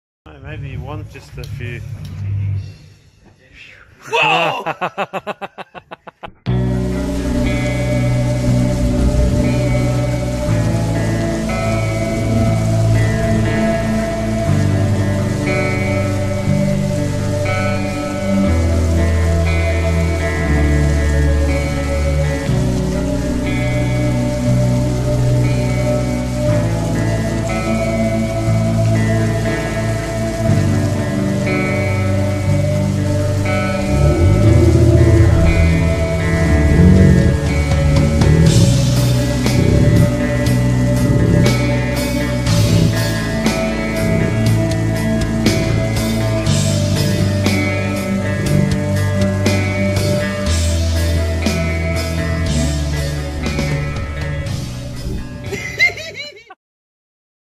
Great Balls of Fire with Magnus Danger Magnus
Magnus (middle name Danger) makes us look up with his vertical hadouken, but sneakily sets the bowl on fire.